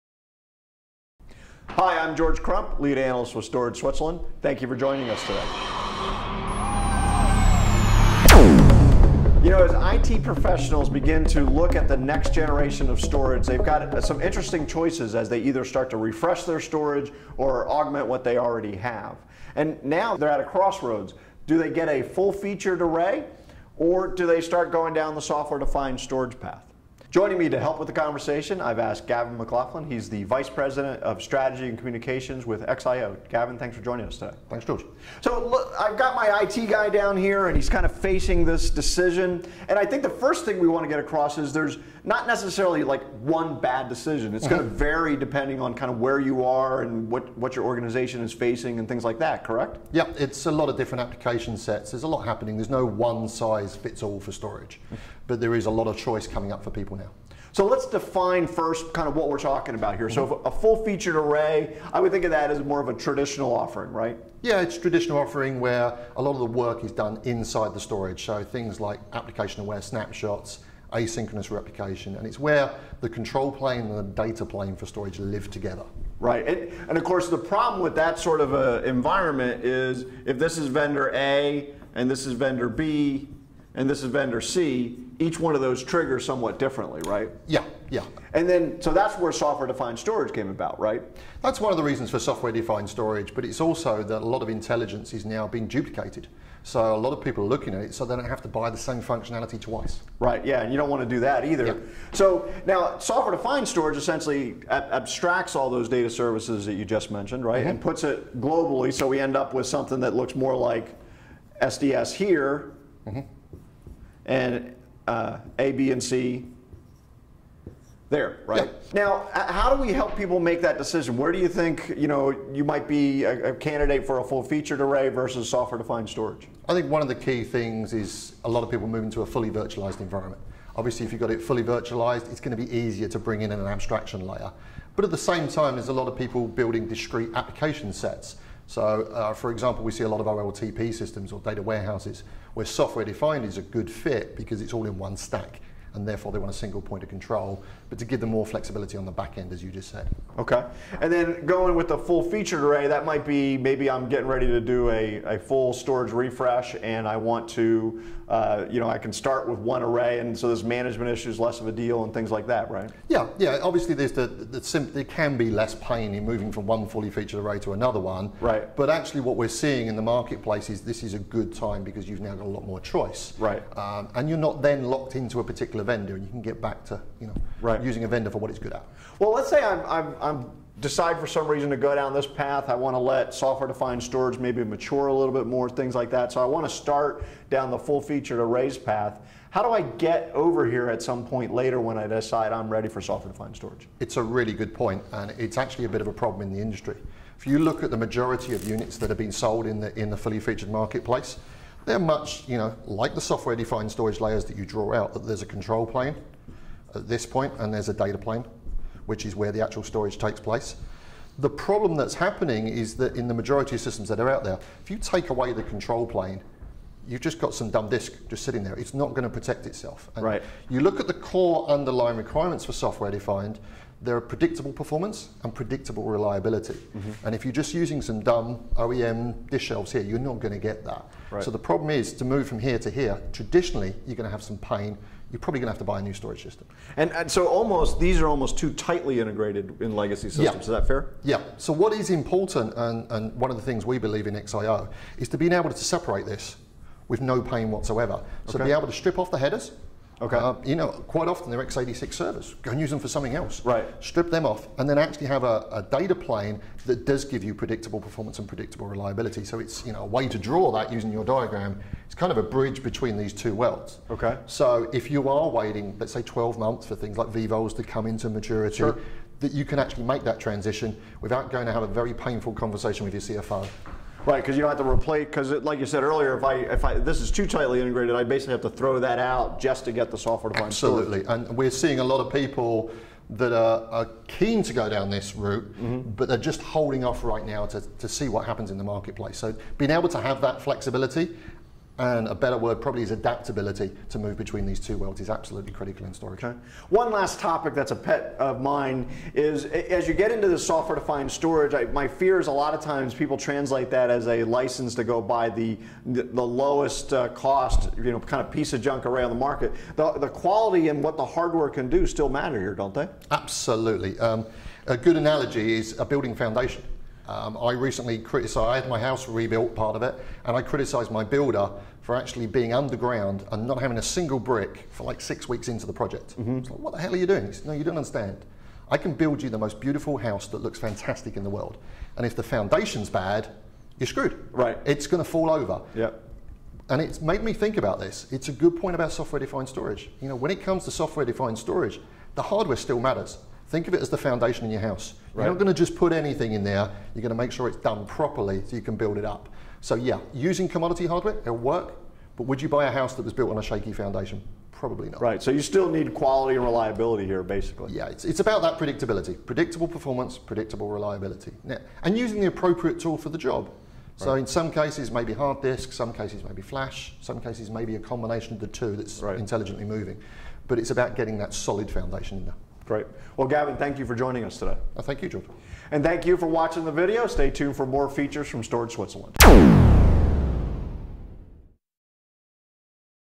Should You Leap to Software Defined Storage (SDS)
As IT professionals begin to refresh or upgrade their storage infrastructure they are faced with a new set of choices during that process. Do they stick with full featured arrays or do they begin investing in software-defined storage. Join Storage Switzerland's Lead Analyst, George Crump and X-IO's VP of Strategy and Communications, Gavin Mclaughlin as they advise IT professionals on how to make the choice that is right for their data center.